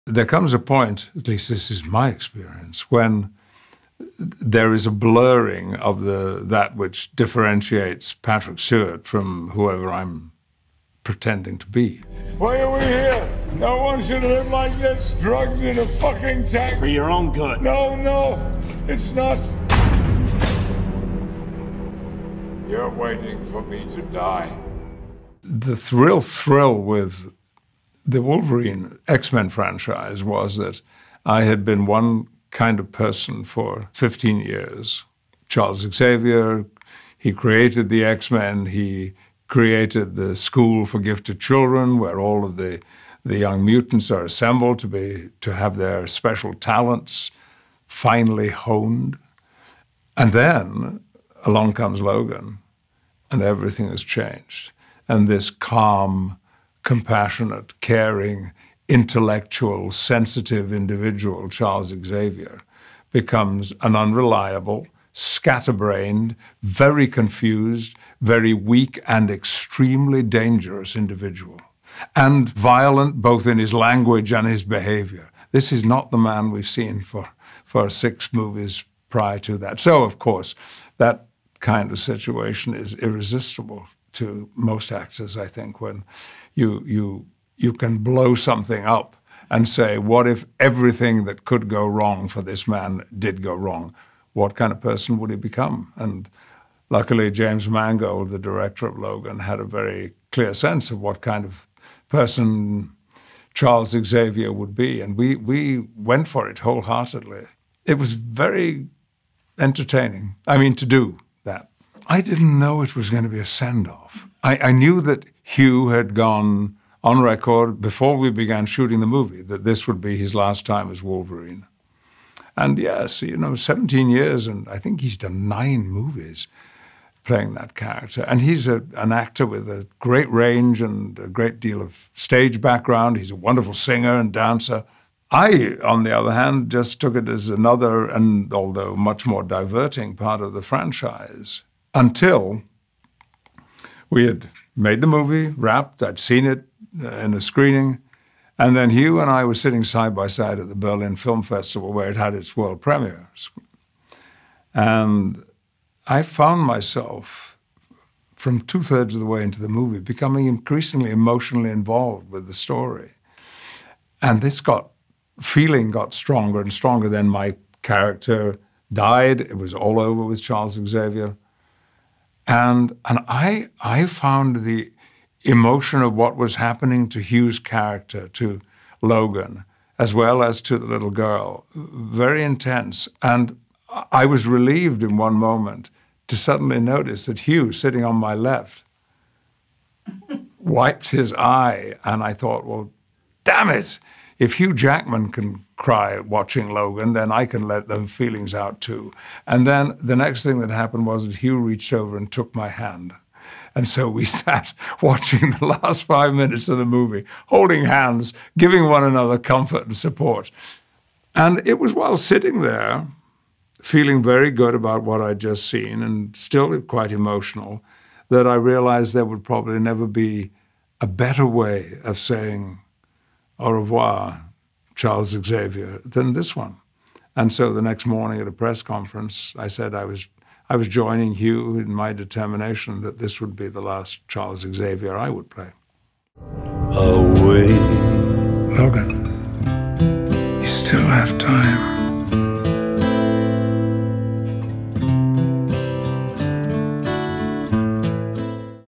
Patrick Stewart on the moment he knew he was done playing Professor X
Sir Patrick Stewart was an integral part of Fox’s X-Men franchise for 17 years as Professor Charles Xavier, but he announced earlier this year he’d be stepping down from the role, after deeming Logan an appropriate farewell. In the video above, he reflects on his time as part of the franchise, and tells us about the moment he knew he was done playing Xavier.